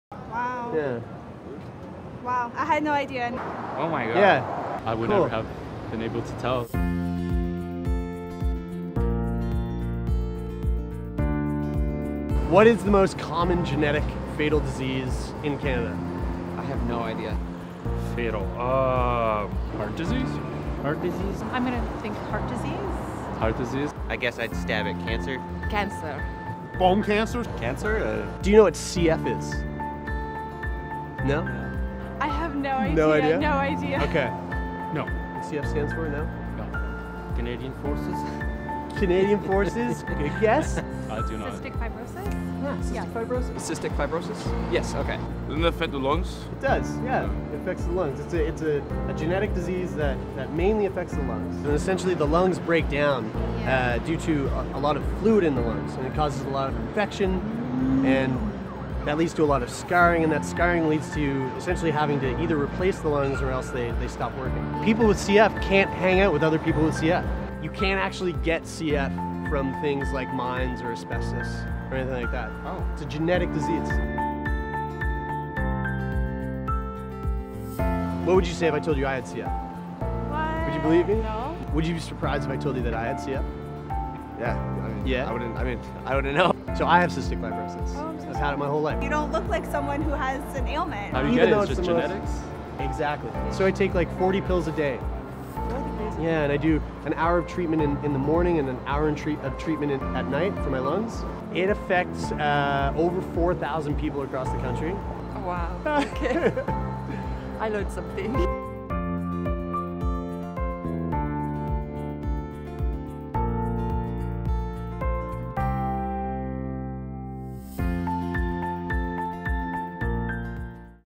What do Canadians know about CF?
It's the most common fatal genetic disease affecting Canadian children & young adults.

Do you know what it is?